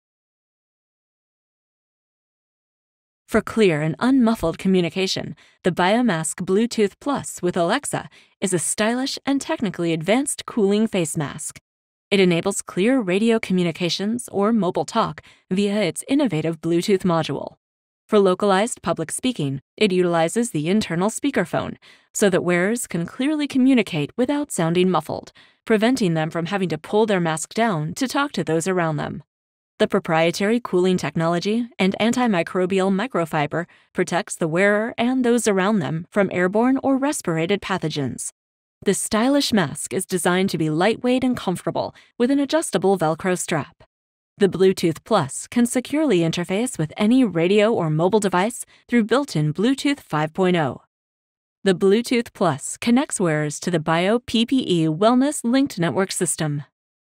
BIOMASK BLUETOOTH+ WITH SPEAKER and MICROPHONE Patent Pending.
Patent Pending Bluetooth Masks with Alexa For Crystal Clear, Unmuffled Communication!

FEATURES
Communicate locally with built-in speaker
Easily make and receive mobile calls
Bluetooth 5.0 technology syncs with any bluetooth device
Internal or mobile app for cooling, made for longtime wear
Light-weight, comfortable, form-fitting material with adjustable velcro strap
Listen to music or podcasts

PURPOSE
An antimicrobial BioMask material barrier between the outside world and the wearer
Safely and clearly communicate to those around you through the built-in localized speakerphone
Commercial and Broad Personal Use (i.e. Public speakers, healthcare workers, legal & law enforcement and the general consumer)